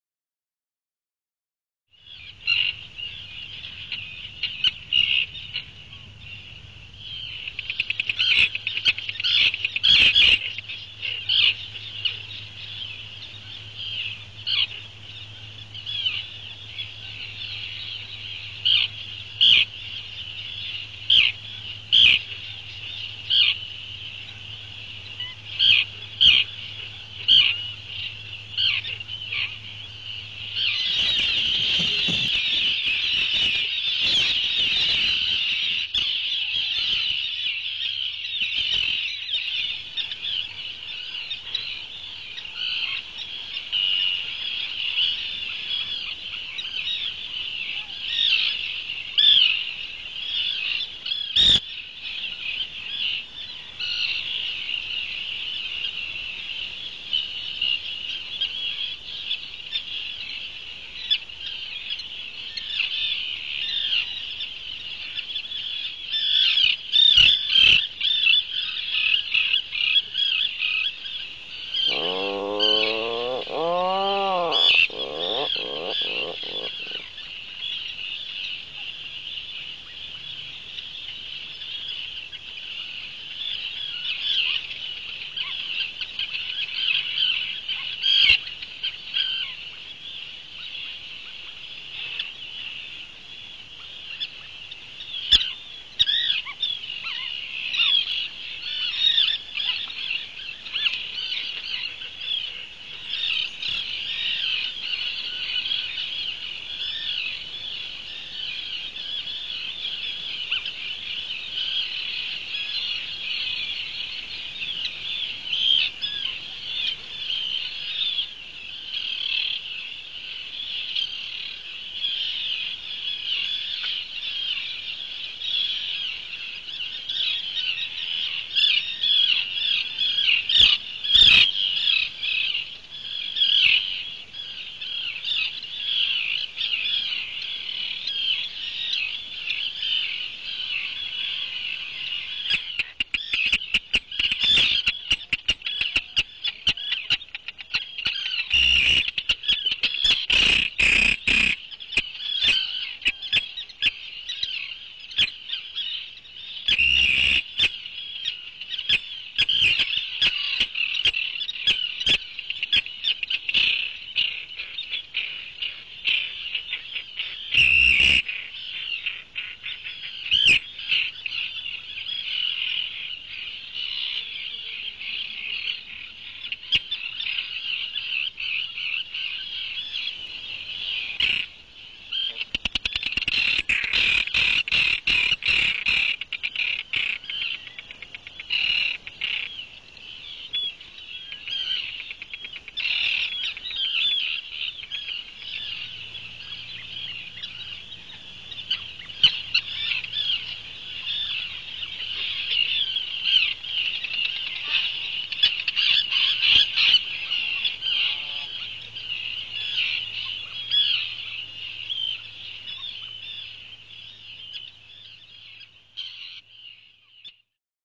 A Puffin Comes Out Of Its Shell
- On June 28th, 2014 our Seal Island Puffin Burrow egg hatched.  Watch the highlights of the egg pipping, a tiny beak breaking through, and the chick fully emerged.  Finally, see the baby's first first meal: a beak-full of fish from its loving parents.